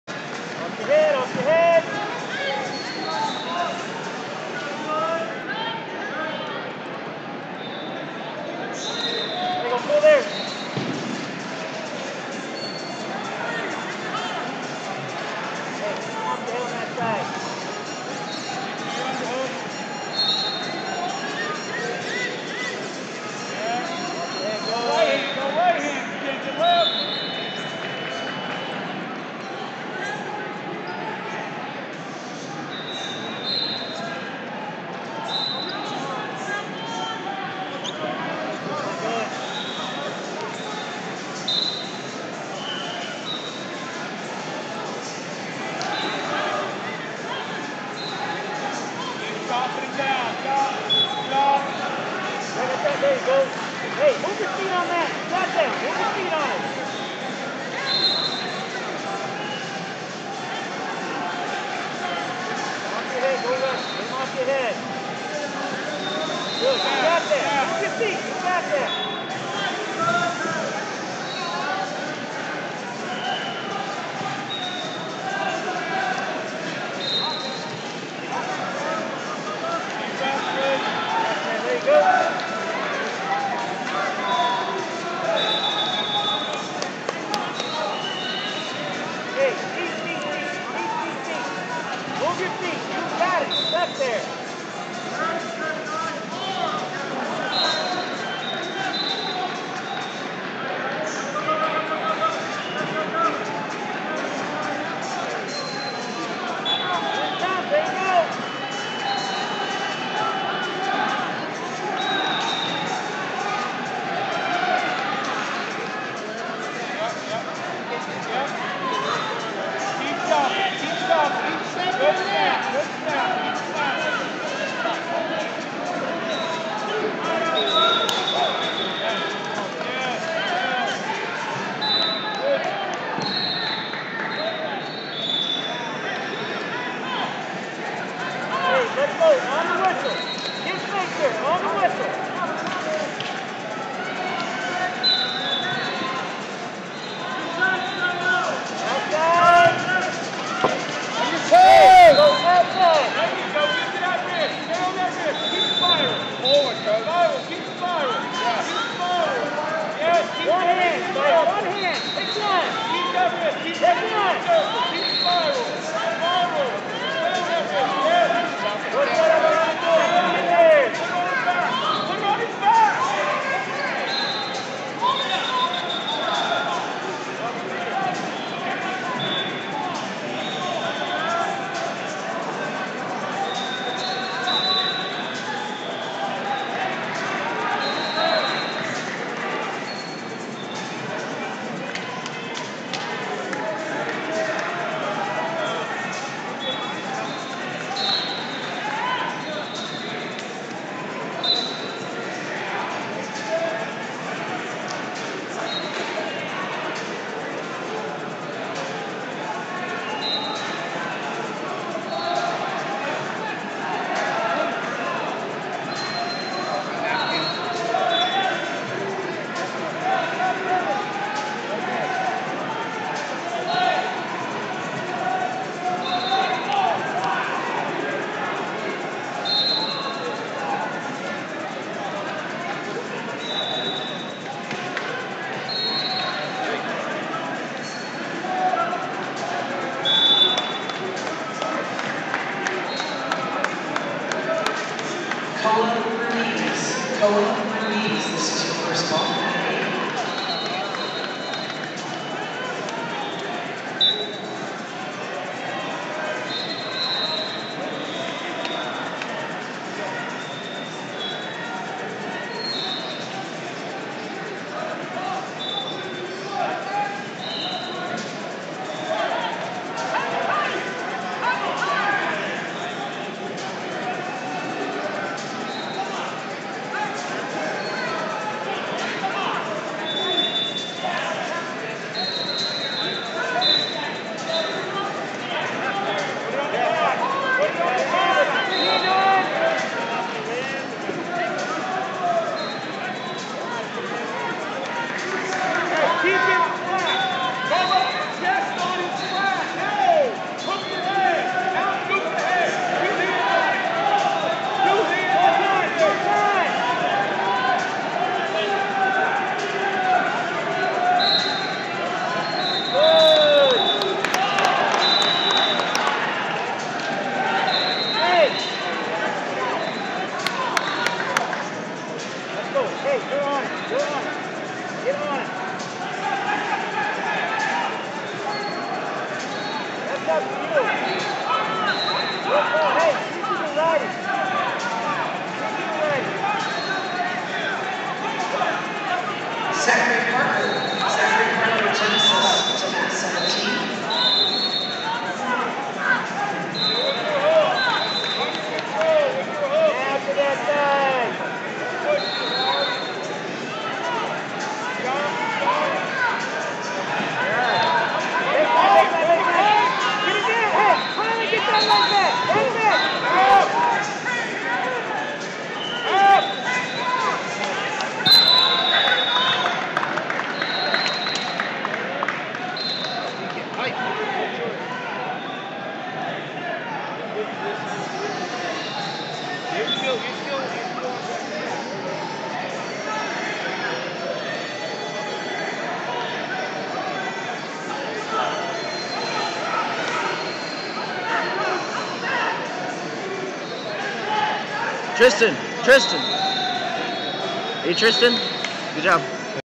2020 USAW Preseason Nationals/1831 High School 11th-12th Grade 220 Oran Varela Unity Youth Wrestli
2020 USAW Preseason Nationals - 2020 USAW Preseason Nationals/1831 High School 11th-12th Grade 220 Oran Varela Unity Youth Wrestling Club Vs Tristan Middlebrook Lake Gibson High School Wrestling